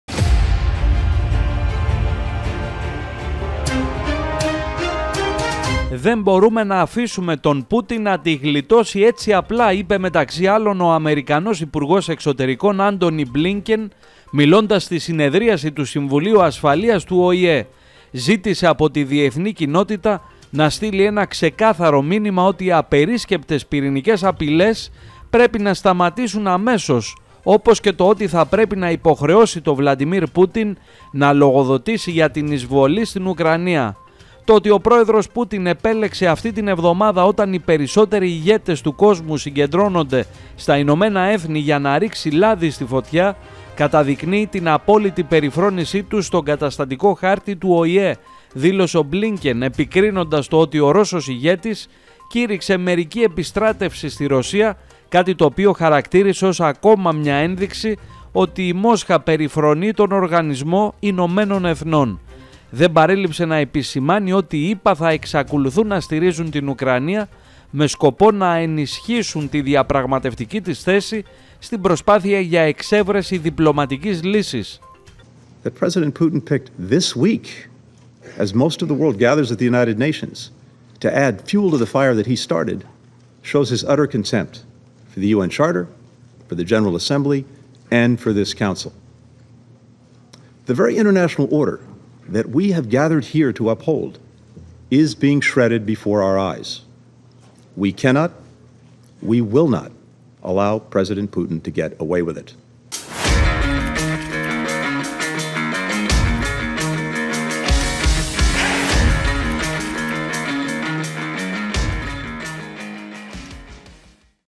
Ο ΠΟΥΤΙΝ ΘΑ ΠΡΕΠΕΙ ΝΑ ΛΟΓΟΔΟΤΗΣΕΙ ΕΙΠΕ Ο ΜΠΛΙΝΚΕΝ ΣΤΟΝ ΟΗΕ
«Δεν μπορούμε να αφήσουμε τον Πούτιν να τη γλιτώσει έτσι απλά», είπε μεταξύ άλλων ο Αμερικανός Υπουργός Εξωτερικών Άντονι Μπλίνκεν  μιλώντας στη συνεδρίαση του Συμβουλίου Ασφαλείας του ΟΗΕ. Ζήτησε από τη Διεθνή κοινότητα «να στείλει ένα ξεκάθαρο μήνυμα ότι οι απερίσκεπτες πυρηνικές απειλές (της Ρωσίας) πρέπει να σταματήσουν αμέσως», όπως και το ότι θα πρέπει «να υποχρεώσει τον Βλαντίμιρ Πούτιν να λογοδοτήσει για την εισβολή στην Ουκρανία».